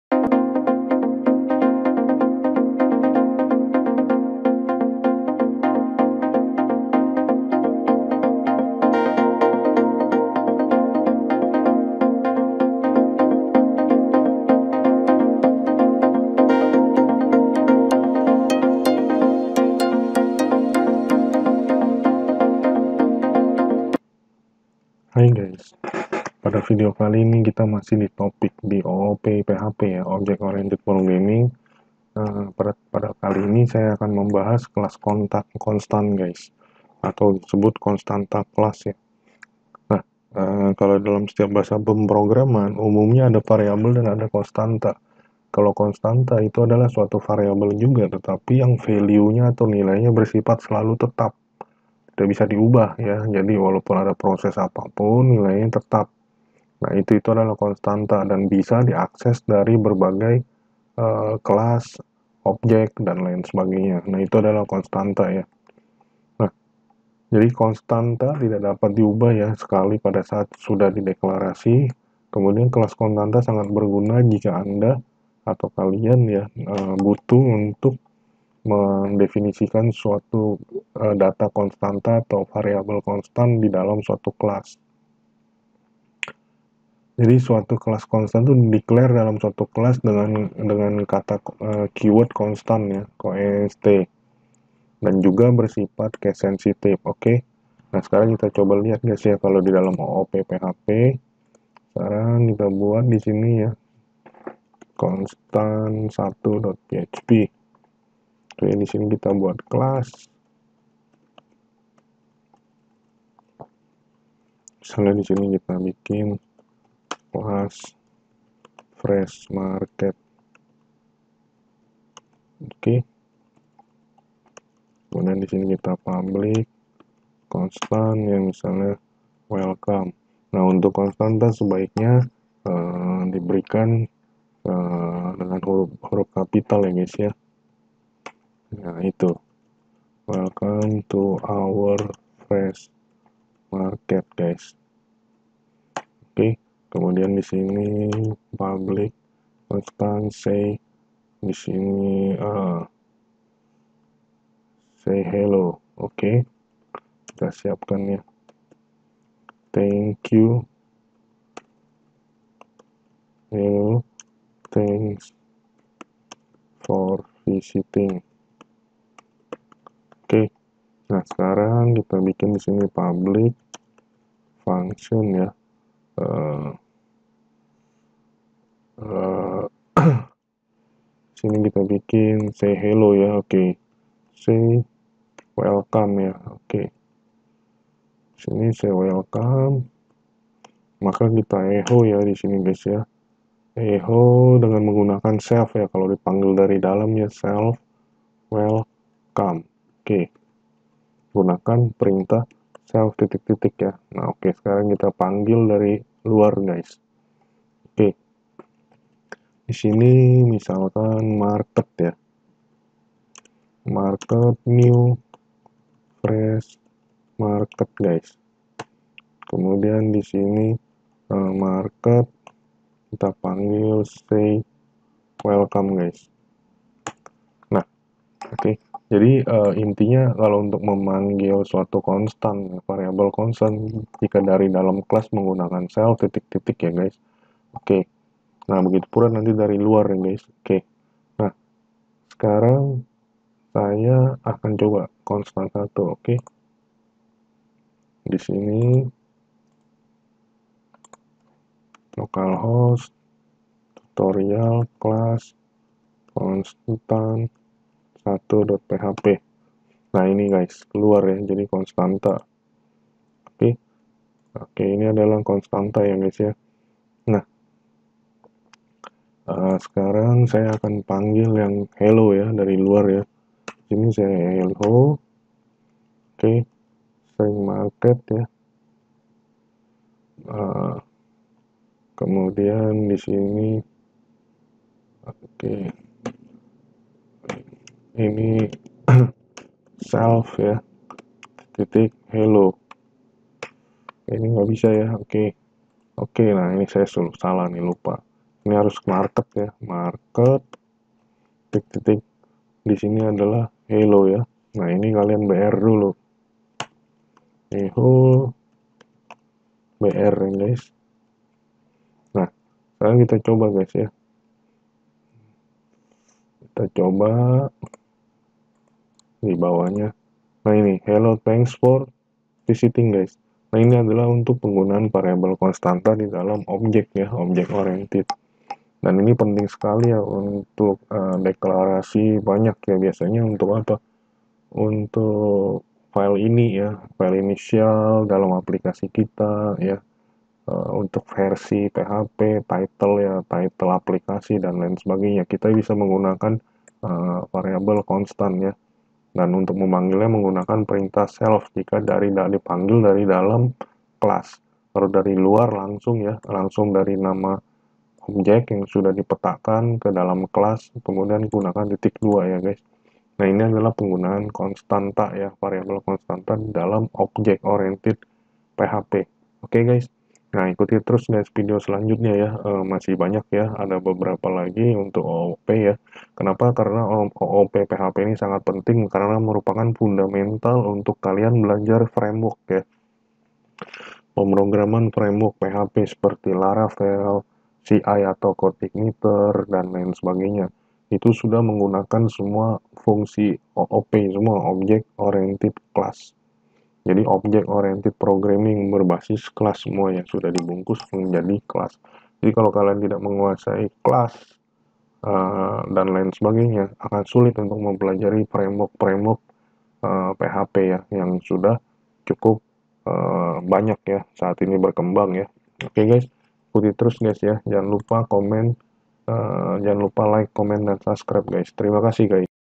09-OOP PHP CLASS CONSTANT
Hi Guys, pada video kali ini akan dijelaskan penggunaan Kelas Konstanta atau Class Constant dalam OOP (object oriented programming) PHP